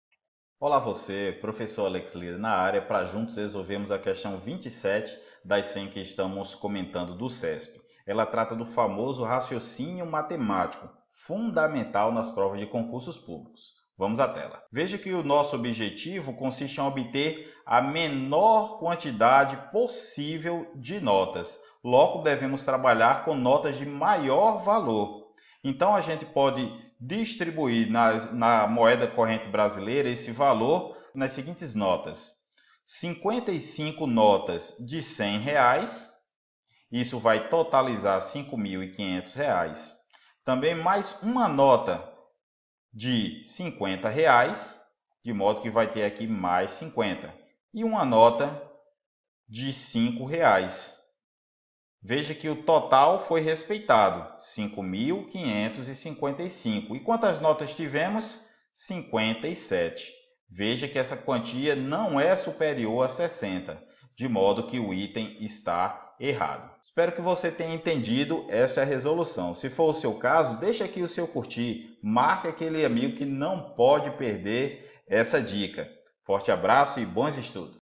Projeto 100 Questões Comentadas da Cespe | RLM #27
O Professor Alex Lira montou o Projeto 100 Questões Comentadas da CESPE para ajudar você a se manter focado em sua prova e ajudar a realizar o seu sonho de se tornar um servidor público.

Gostou do vídeo? Deixe o seu comentário e siga a página do Exponencial!"